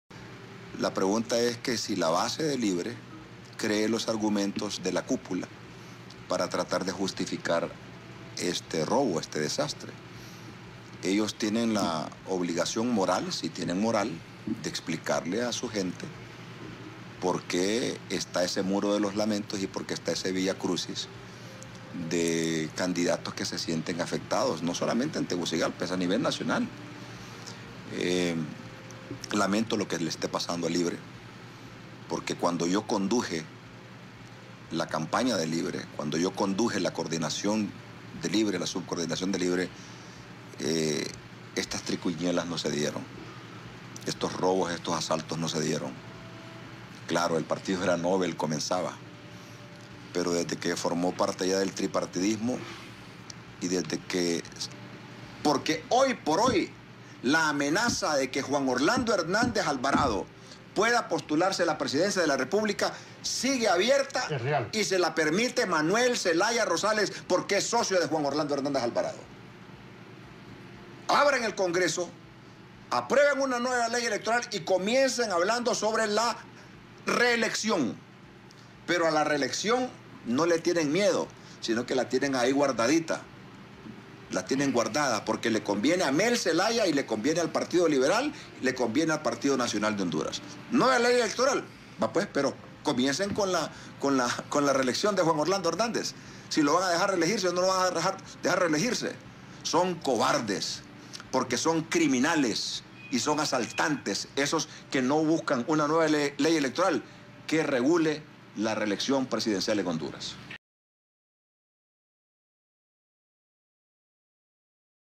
Esdras Amado López arremete contra Manuel Zelaya Rosales.
ElPulso CuartoPoder | El periodista y candidato presidencial arremetió contra Manuel Zelaya Rosales a quien responsabiliza del fraude electoral en las elecciones primarias de Libre además aseguró que es socio de Juan Orlando Hernández.